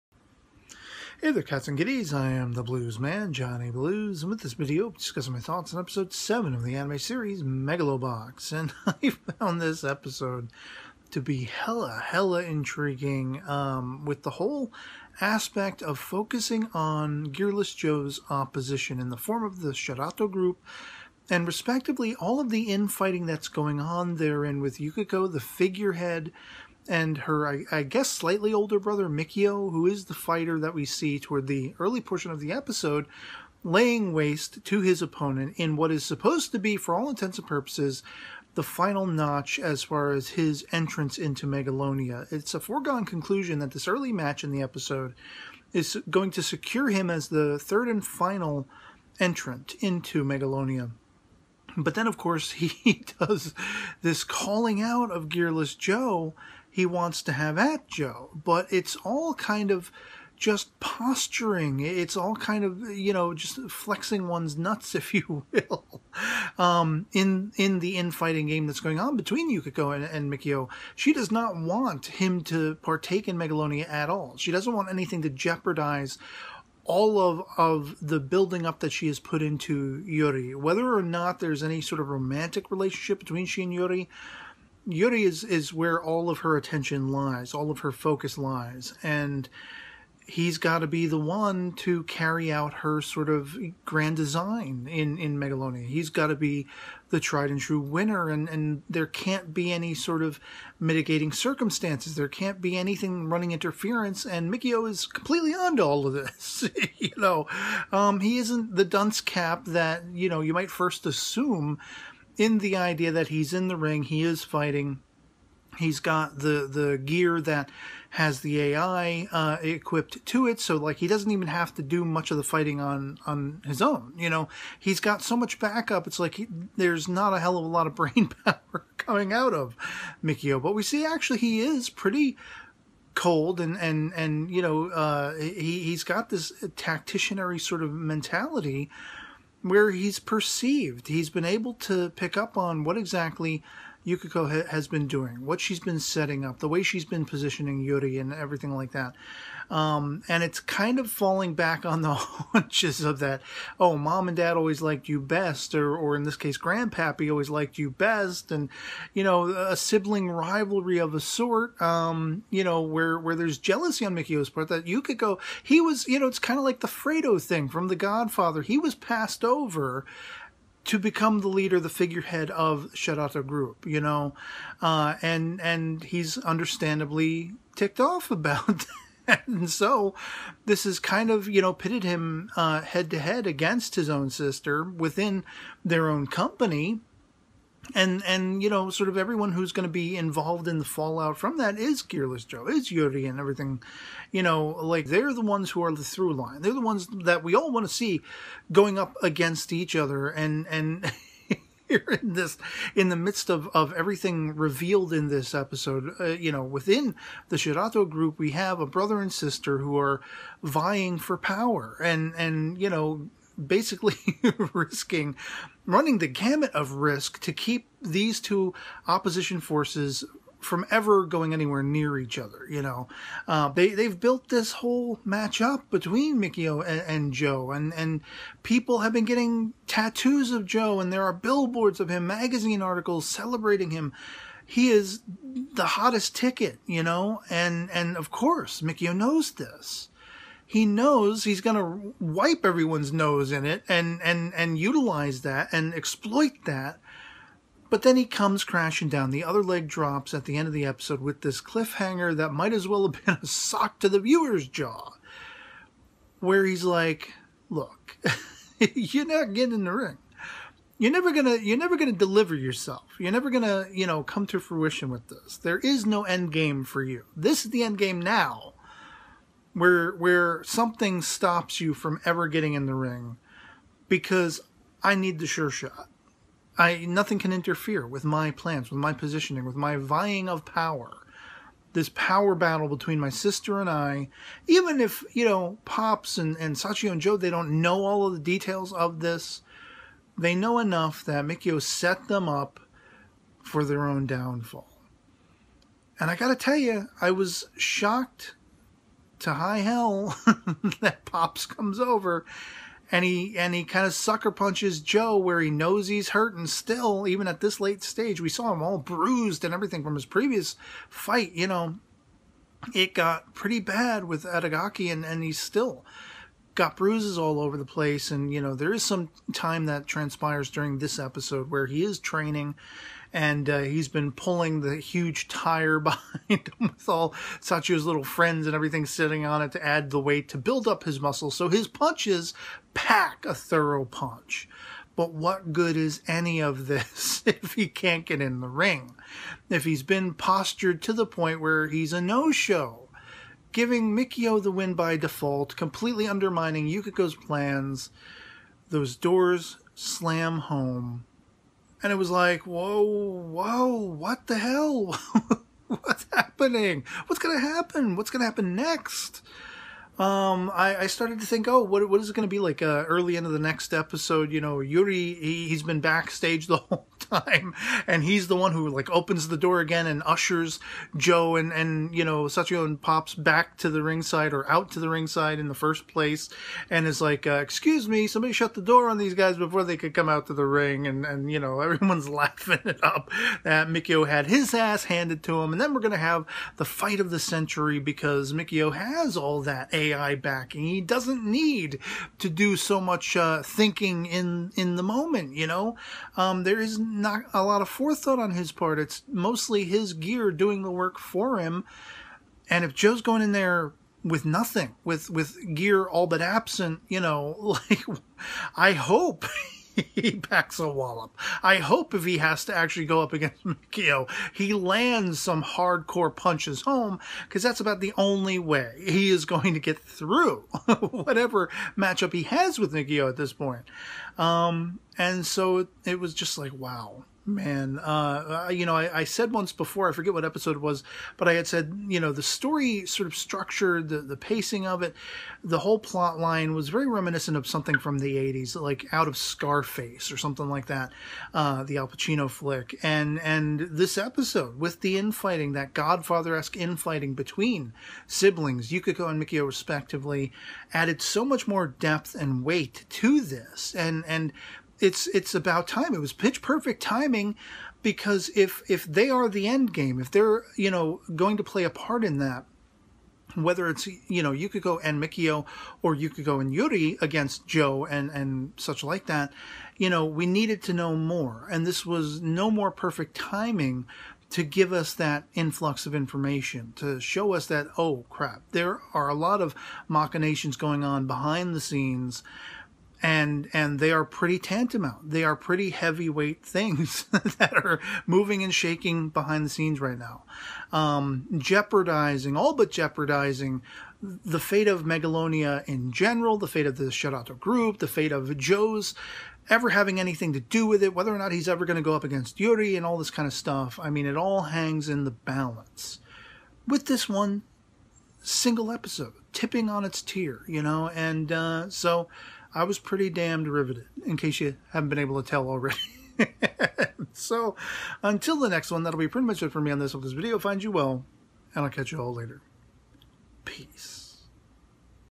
Megalo Box Episode 7 メガロボクス - Review
Discussing my thoughts on episode 7 of the anime series, Megalo Box... What say you?